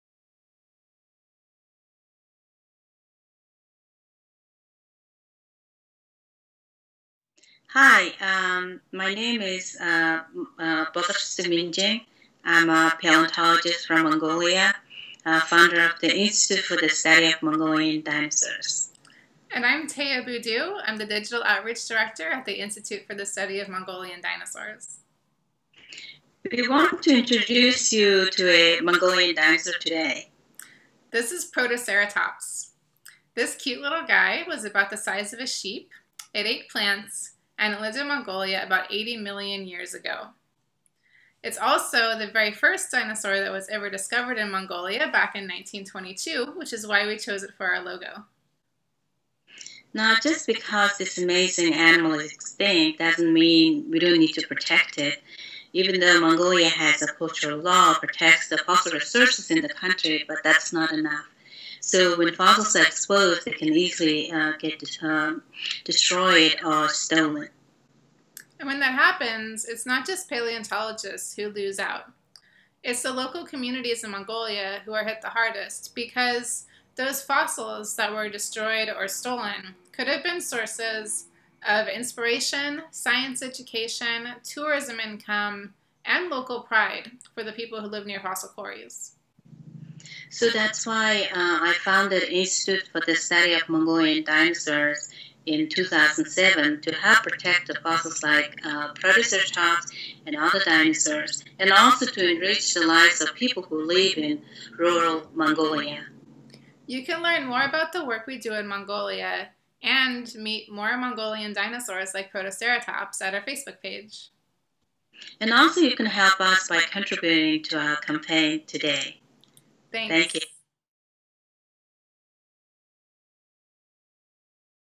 Meet a Mongolian Dinosaur
The Institute for the Study of Mongolian Dinosaurs
presents
A message from our Founder in New York
and Digital Outreach Director in San Francisco

Монголын Динозавр Судлалын Хүрээлэн
Мэндчилгээ
Үүсгэн Байгуулагч - Нью Йорк хотоос,
Цахим Боловсролын Захирал - Сан Франциско хотоос

Bolor:
Hi! My name is Bolortsetseg Minjin and I am a paleontologist from Mongolia, and the founder of the Institute for the study of Mongolian Dinosaurs.
Сайн байна уу, Миний нэрийг Минжингийн Болорцэцэг гэдэг.
Палеонтологч мэргэжилтэй.
Монголын Динозавр Судлалын Хүрээлэн ТББ үүсгэн байгуулагч.

Thea:
And I’m Thea Boodhoo, director of digital outreach.
Намайг Тэя Бѳѳдхүү гэдэг.
  Тус хүрээлэнгийн цахим танин мэдэхүйн боловсролын захирал.

Bolor:
We want to introduce you to a Mongolian dinosaur today.
Ѳнѳѳдѳр та бүхэнд бид Монголын нэгэн динозаврыг танилцуулья.

Thea:
This is Protoceratops, a dinosaur related to Triceratops that lived in Mongolia 80 million years ago. It was a plant-eater about the size of a sheep. And it was the first dinosaur discovered in Mongolia, which is why we chose it for our logo.
Энэ бол Протоцератопс.
Биеийн хэмжээ нь хонинтой ойролцоо.
Ѳвсѳн тэжээлтэн, 80 сая жилийн ѳмнѳ Монголд амьдарч байсан.
Монголоос олдсон хамгийн анхны динозавр,
тиймээс бид байгууллагынхаа бэлэг тэмдэг болгосон.

Bolor:
Now just because this awesome animal is extinct doesn’t mean we don’t need to protect it. Dinosaur fossils are protected by cultural law in Mongolia, but there aren’t many resources to actually protect them in the wild. So exposed fossils can be easily destroyed or stolen.
Энэ динозавр сѳнѳсѳн гээд бид хамгаалахгүй байж болохгүй.
Монголын соёлын ѳвийн хуулиар хамгаалагдсан ч гэсэн энэ нь хангалттай биш юм.
Газрын гадарга дээр ил гарсан динозавр эвдэрч эсвэл хулгайлагддаг.

Thea:
And when that happens, it’s not just paleontologists who lose. It’s rural communities in Mongolia that take the biggest hit, because fossils that are stolen or destroyed could have been a source of inspiration, science education, tourism income, and local pride for the people who live near the quarries.
Ингэснээр, палеонтологчид хохирохоос гадна орон нутгийнханд их хохиролтой.
Олдвор эвдэрч, хулгайлагдснаар шинжлэх ухааны сонин сайхан нээлт, танин мэдэхүй,
аялал жуучлал болон орон нутагийн бахархал үгүй болж байна гэсэн үг юм.

Bolor:
That’s why I founded the Institute for the Study of Mongolian Dinosaurs in 2007. To help protect the fossils of Protoceratops and other dinosaurs–and to enrich the lives of people who live in rural Mongolia.
Тиймээс 2007 онд миний бие Монголын Динозавр Судлалын Хүрээлэн ТББ үүсгэн
байгуулж Протоцератопс шиг бусад динозаврын олдворуудыг хамгаалахад,
мѳн орон нутагт хүмүүсийн амьдралыг сайжруулахад хувь нэмрээ оруулах зорилготой.

Thea:
You can follow our work and meet more Mongolian dinosaurs on our Facebook page
Манай Нүүрномноос та бүхэн нэмэлт мэдээлэл авч, Монголын динозавруудтай танилцаарай.
Bolor:
And you can help us by contributing to our campaign today.
Бидний ажлыг дэмжиж, хандив босгох аянд маань нэгдээрэй.

Thea & Bolor:
Thanks!
Баярлалаа!